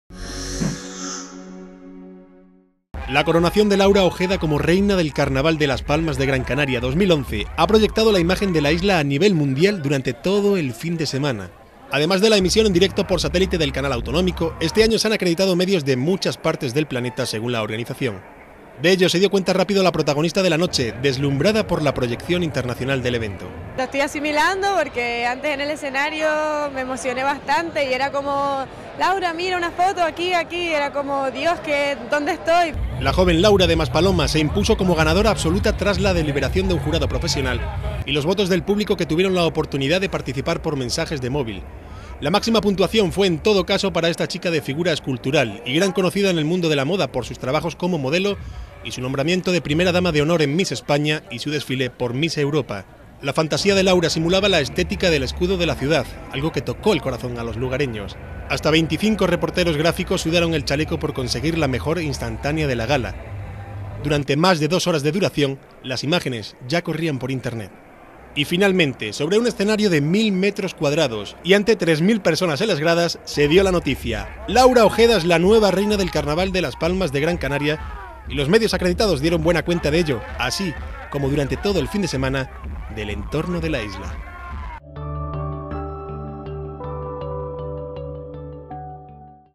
Gran afluencia de medios en el Carnaval de Las Palmas de Gran Canaria 2011
Laura Ojeda con la fantasia 'Soy Gran Canaria', de Fernando Méndez, ha sido la sirena elegida para reinar en los festejos de Las Palmas de Gran Canaria 2011. El evento contó nuevamente con gran afluencia de medios y de periodistas acreditados.